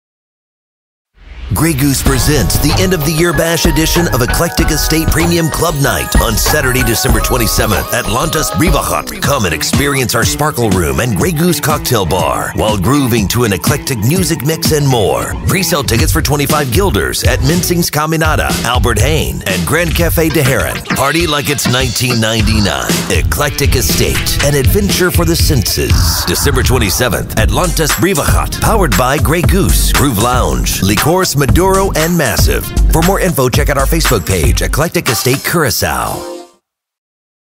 Eclectic Estate end of the Year Bash
The end of the year is near. Are you looking for an extraordinary night out with friends? Then you do not want to miss the End of the Year Bash edition of Eclectic Estate on Saturday, December 27th.

It promises to be a memorable evening starting with acoustic live music and exclusive art. You may treat yourself in our Sparkle room and the Grey Goose cocktail bar while grooving on an eclectic music mix ranging from funk to future sounds mixed by top club DJ’s. All this set in a ‘let’s Party like its 1999’ ambiance at the beautiful scenery of Landhuis Brievengat.

Eclectic Estate,
an adventure for the senses.

Pre-Sale FL 25, -
Ticket Sales: Mensing Caminada, Albert Heijn and Grand Café De Heeren.
Date: Saturday, December 27th
Time: Starts 21:00
Place: Landhuis Brievengat
Talent: Mr. Cash, Eli, Enrico, Wynsel, Joey B, Lovely & DJ TICO